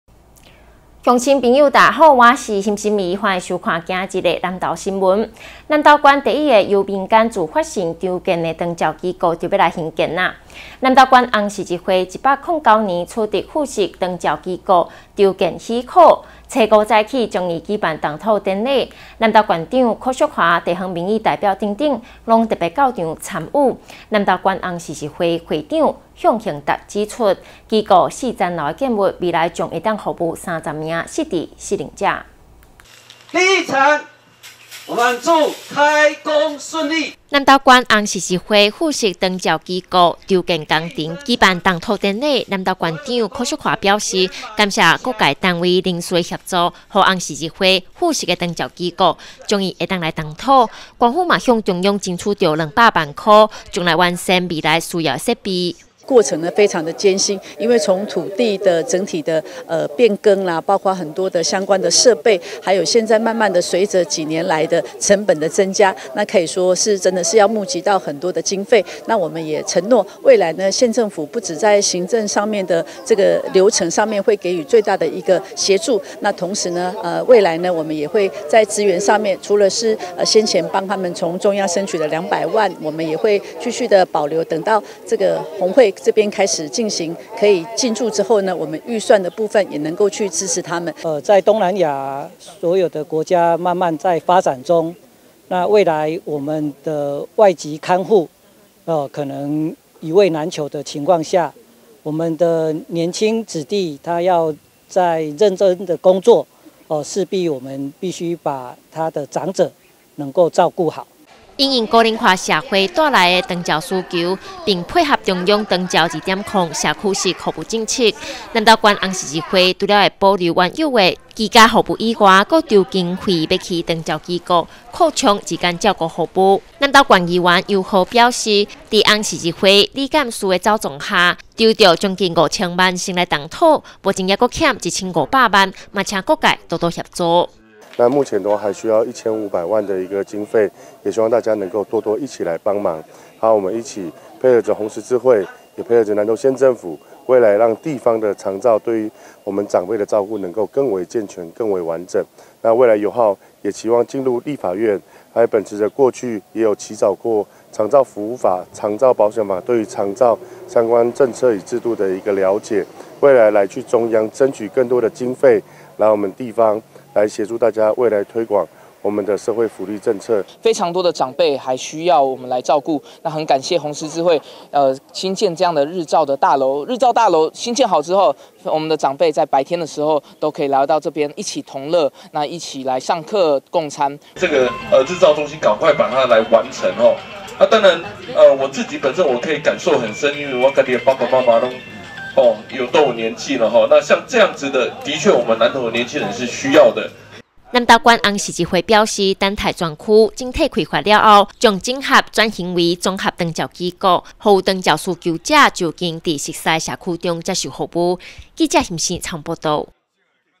民間自發性籌建 南投縣紅十字會附設長照機構動土 南投新聞
南投縣第一個由民間自發性籌建的長照機構，即將興建！南投縣紅十字會，109年取得附設長照機構籌建許可，5日上午終於舉辦動土典禮，南投縣長許淑華、地方民意代表等人，都特別到場參與。南投縣紅十字會長向恒達指出，機構4層樓建物，未來將可服務30名失智、失能者。